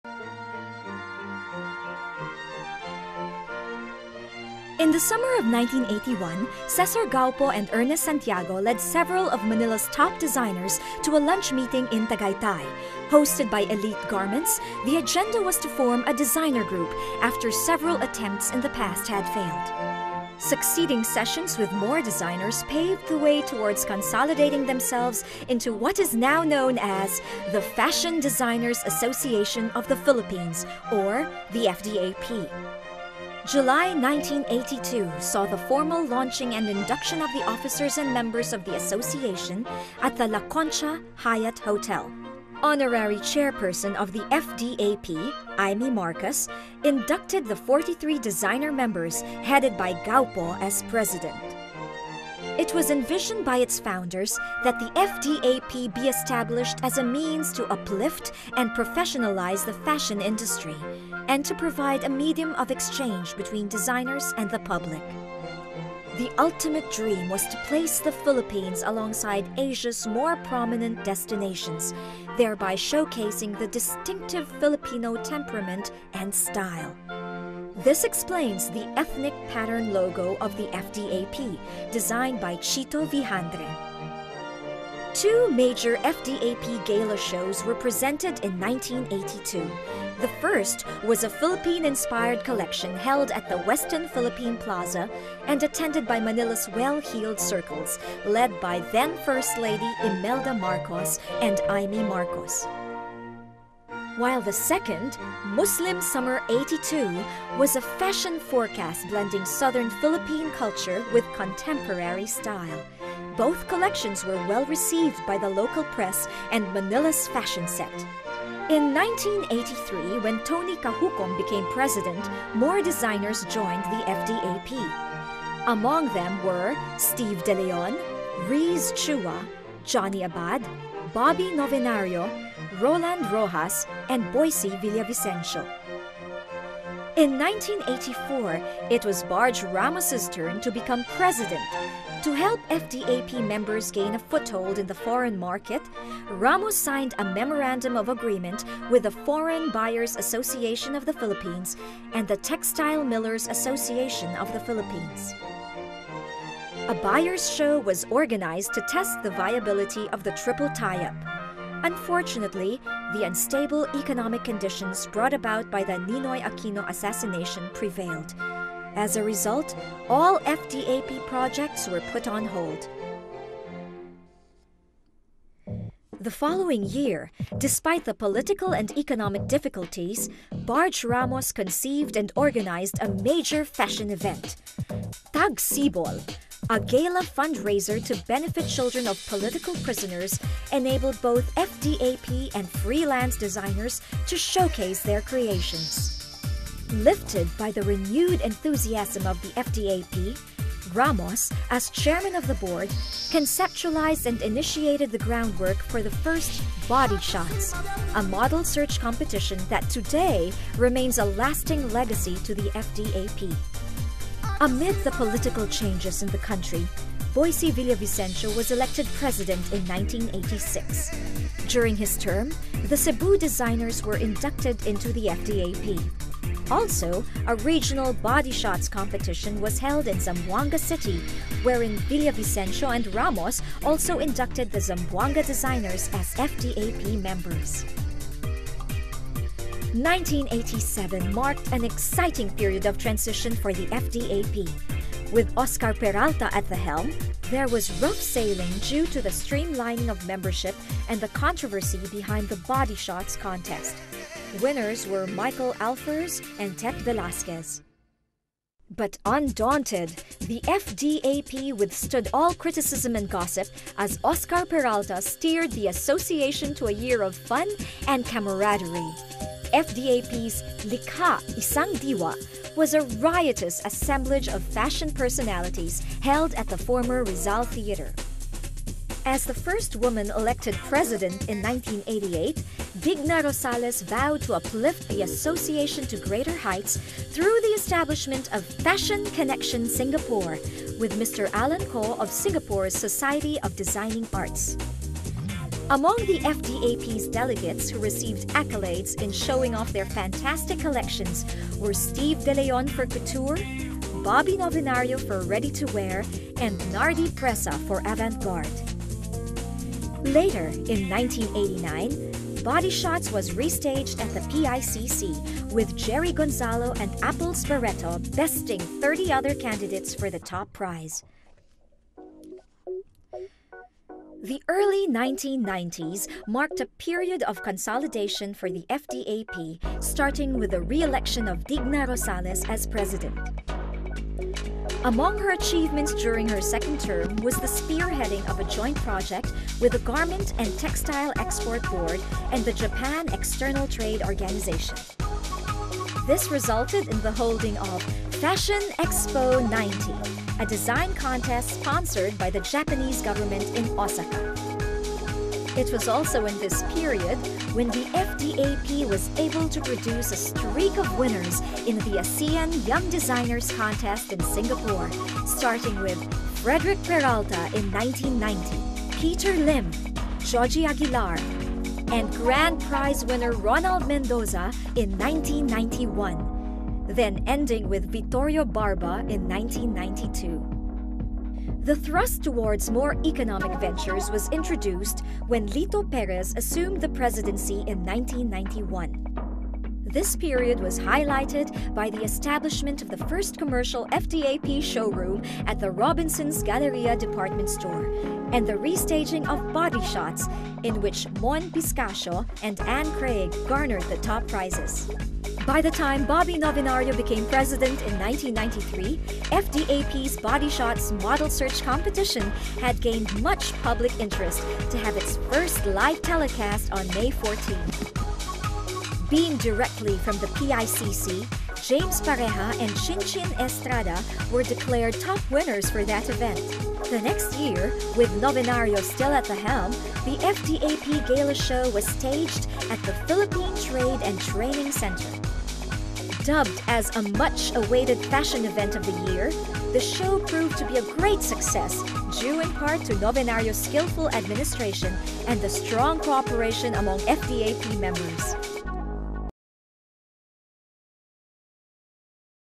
The History of the FDAP (Part 1)
This is the definitive history of the Fashion Designers Association of the Philippines or FDAP.

Commissioned by 3-time FDAP President Edgar San Diego (one of the original members) on the occasion of their 25th Anniversary in 2006

Voiceover by Liesl Batucan